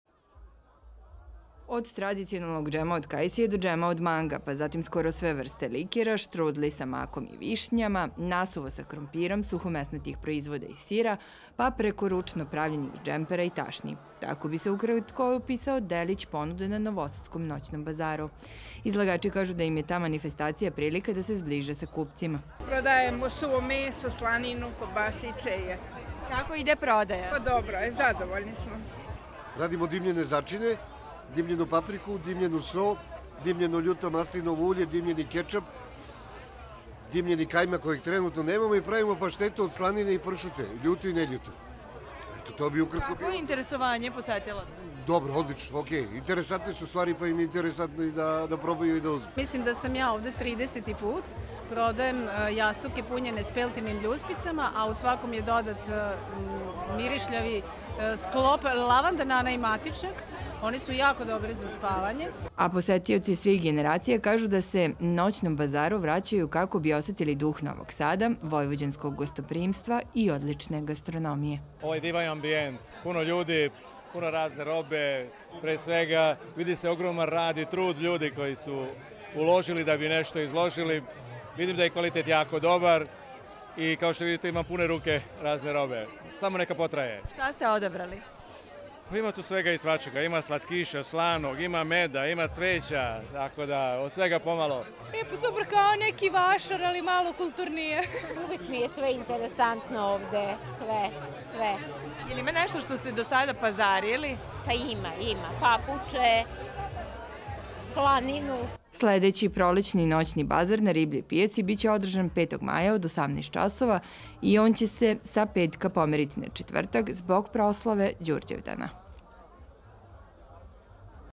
Noćni bazar na Ribljoj pijaci
I ovoga petka novosadska Riblja pijaca centar je okupljanja malih proizvođača, zanatlija i preduzetnika, koji su dobili mogućnost da se predstave sa svojim proizvodima posetiocima Noćnog bazara. Ta tradicionalna manifestacija je večeras održana po jedanaesti put.